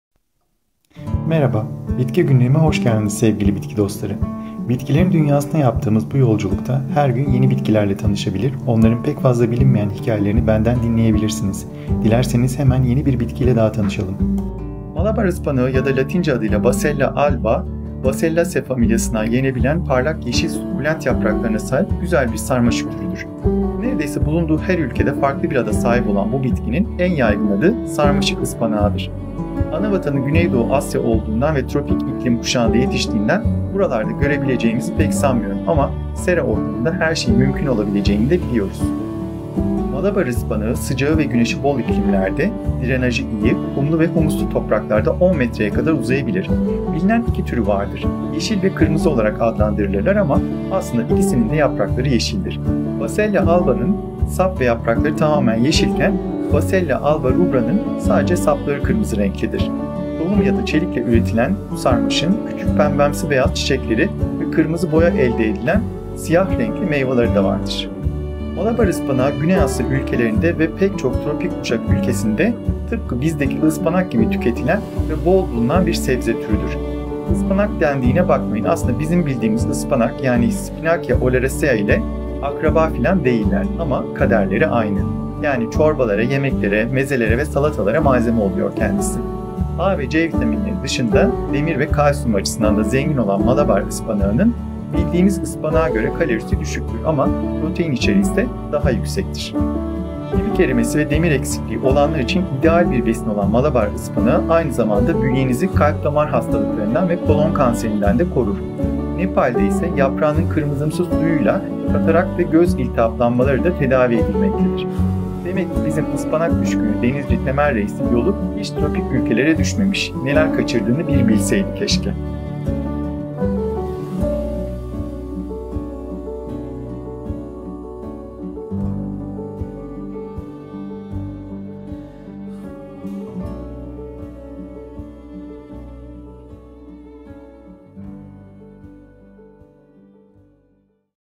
Malabar ıspanağı - Basella alba
"Malabar ıspanağı, sıcağı ve güneşi bol iklimlerde, drenajı iyi, kumlu ve humuslu topraklarda 10 metreye kadar uzayabilir. Bilinen iki türü vardır: yeşil ve kırmızı olarak adlandırılırlar ama aslında ikisinin de yaprakları yeşildir. Basella alba’nın sap ve yaprakları tamamen yeşilken, Basella alba rubra’nın sadece sapları kırmızı renklidir. Tohum ya da çelikle üretilen bu sarmaşığın küçük pembemsi beyaz çiçekleri ve kırmızı boya elde edilen siyah renkli meyvaları da vardır. Malabar ıspanağı güney Asya ülkelerinde ve pek çok tropik kuşak ülkesinde tıpkı bizdeki ıspanak gibi tüketilen ve bol bulunan bir sebze türüdür."
Okan Akkoyunlu
Videodaki görseller: Yandex